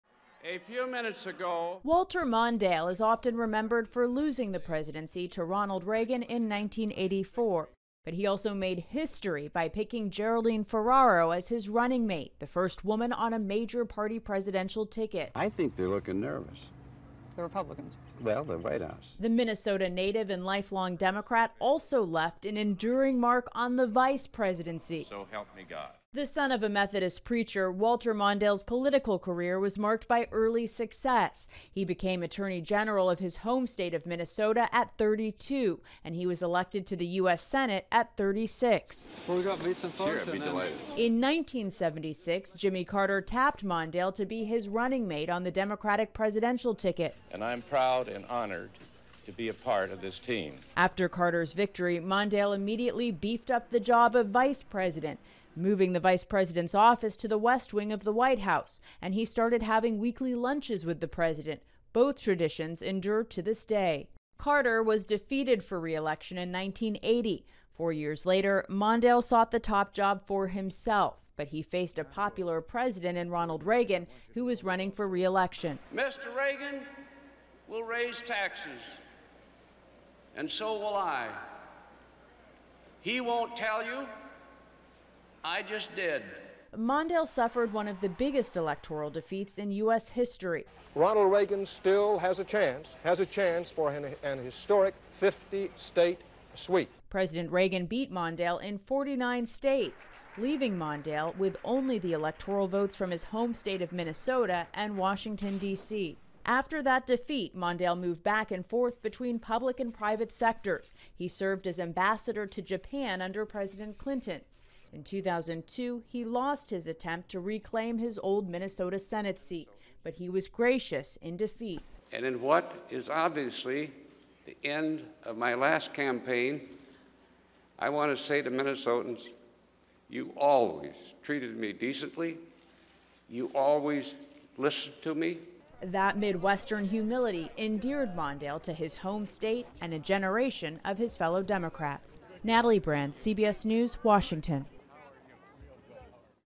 Former Vice President Walter Mondale has died at 93
Former Vice President and presidential candidate Walter Mondale has died at age 93. His family announced he died Monday in Minneapolis. CBS News' Natalie Brand has more on Mondale's ground-breaking political legacy.